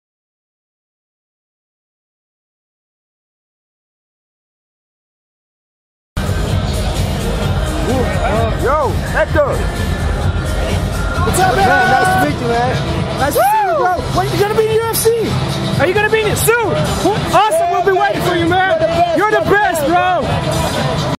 Hector Lombard soon in UFC
We met Hector Lombard at the last nights UFC Sweden. When asked when we are going to see him in UFC he answered with a short "Soon"!